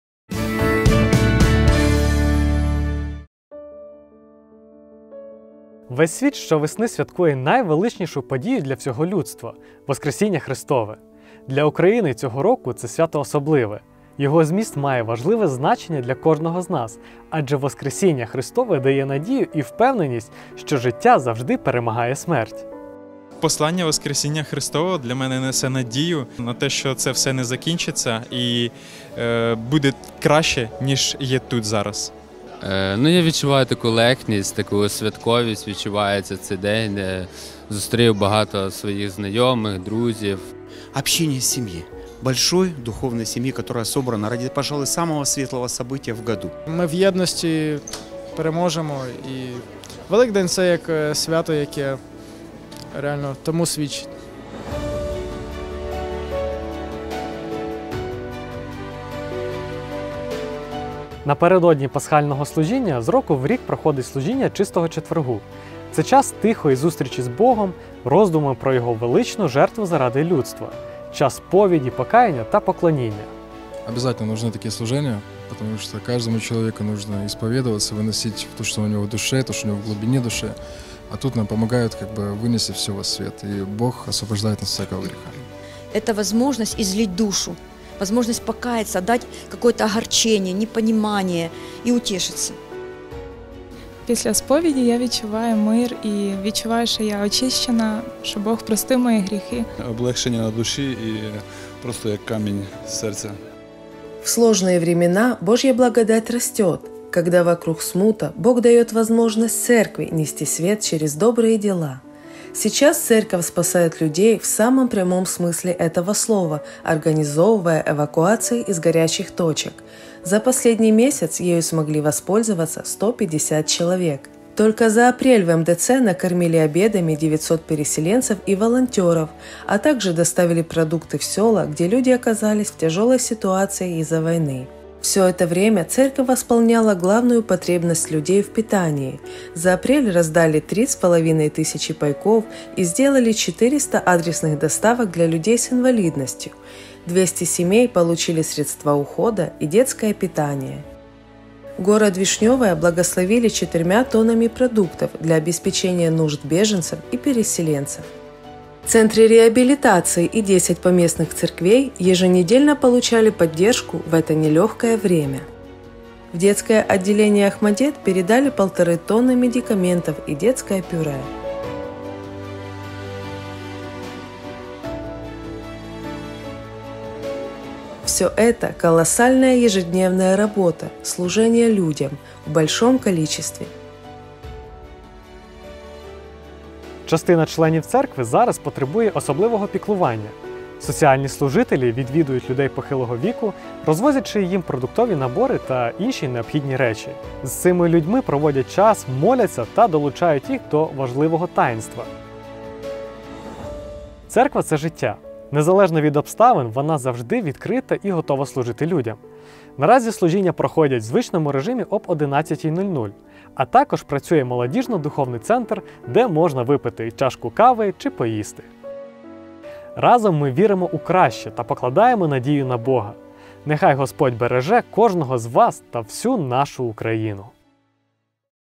Новини церкви "Спасіння" за квітень
Чим жила церква "Спасіння" у квітні?
Як пройшла Пасха цьогоріч?
Для чого людям Чистий четвер?
Як наша церква служить людям під час війни?
Про все це розповідаємо у квітневому випуску новин "Спасіння" ⬆️